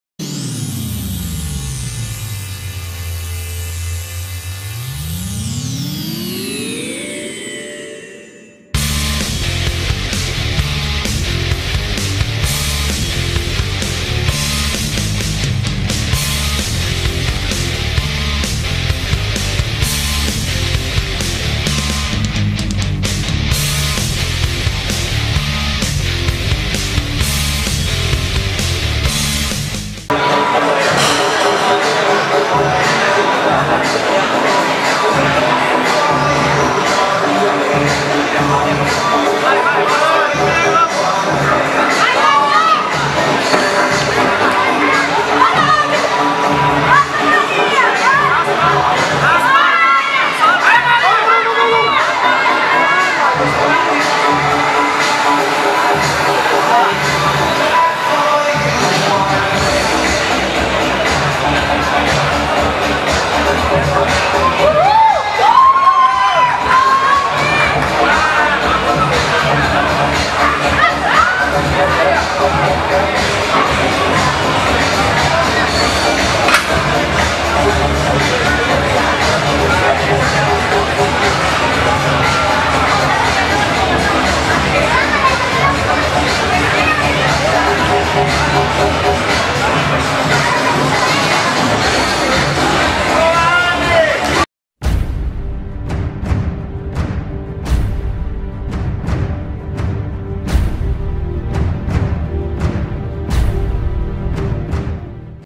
Maria vs Anne - Expo Fight Betim
Anne Yasmin (Titan) venceu Maria de Lourdes (FT BH) por desclassificação